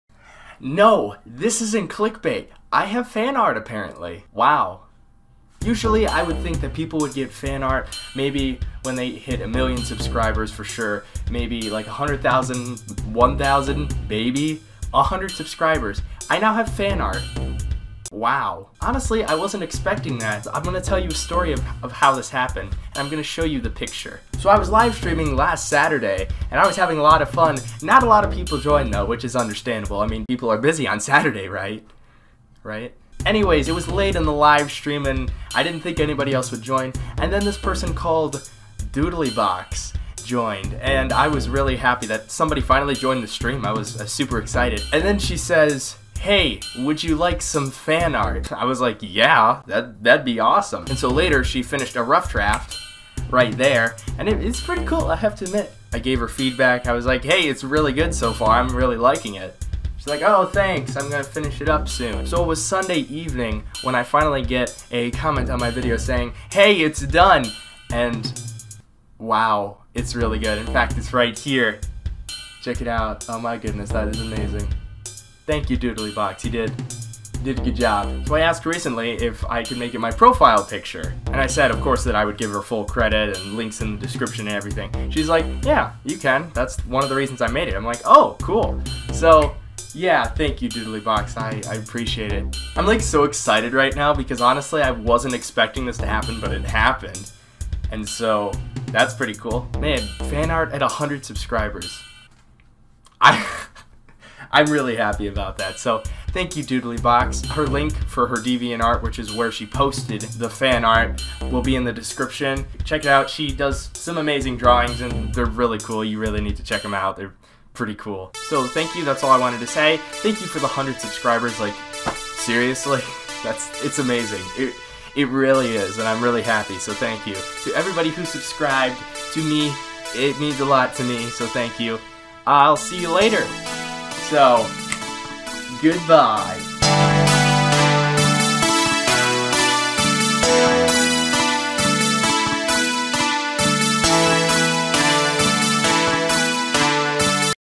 SOMEONE MADE FAN ART OF ME
Honestly, I never thought this would happen. I mean, I'm only at 100 subscribers, and someone made fan art of me THIS SOON??? IT'S CRAZY!!! XD